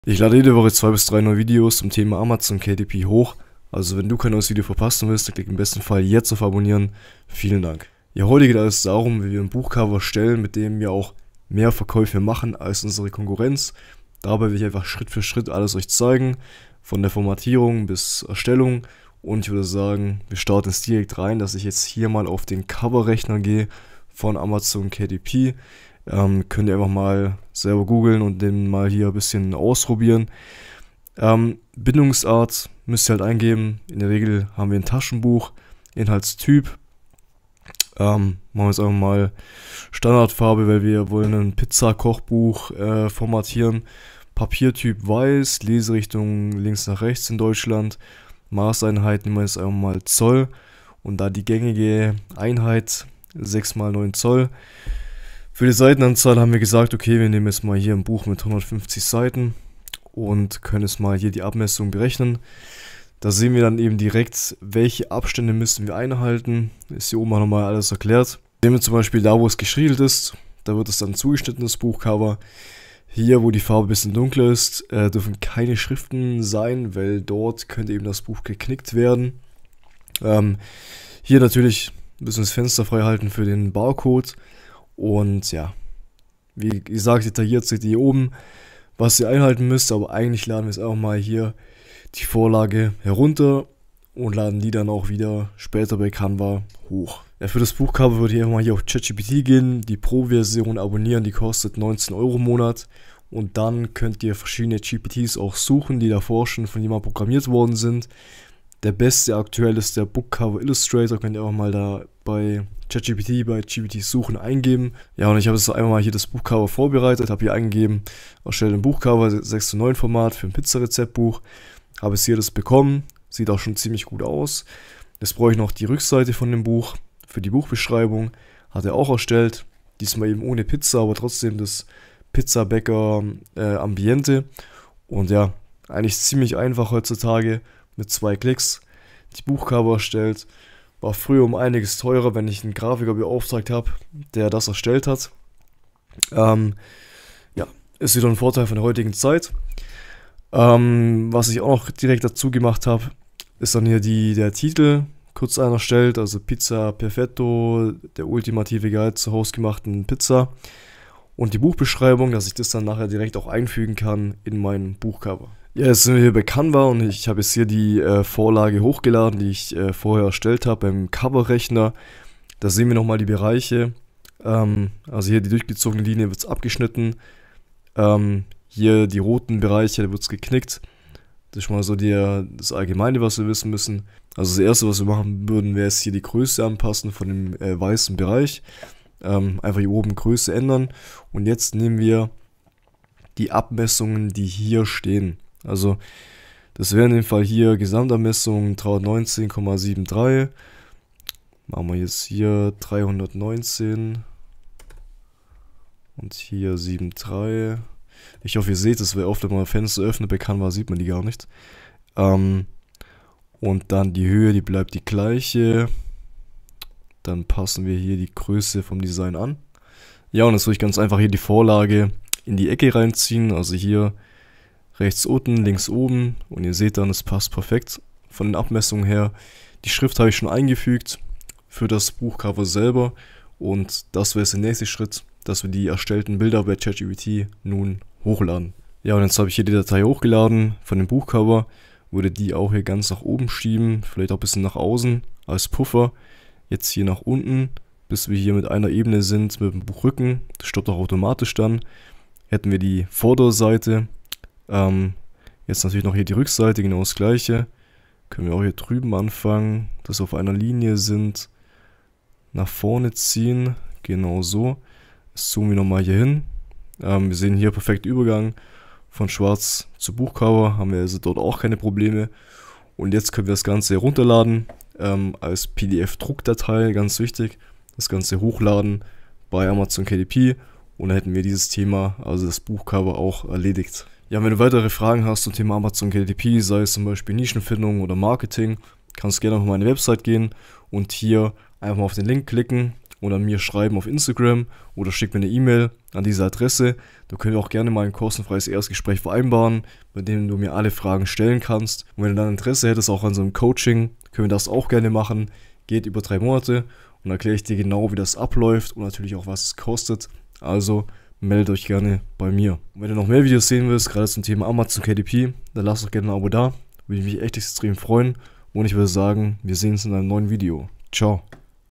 Perfektes KDP-Buchcover: So ziehst du mehr Käufer an!
*Nr. 1 Nischen-Tool HELIUM 10 für KDP

Heute zeige ich euch, wie ihr online Geld verdienen könnt und das ganz einfach mit Amazon KDP. Probiert es doch aus und viel Spaß mit dem Video!

00:00 Einleitung
00:08 Vorlage
01:37 Covererstellung
02:45 Formatierung
04:44 1:1 Coaching
05:13 Gerne Abonnieren!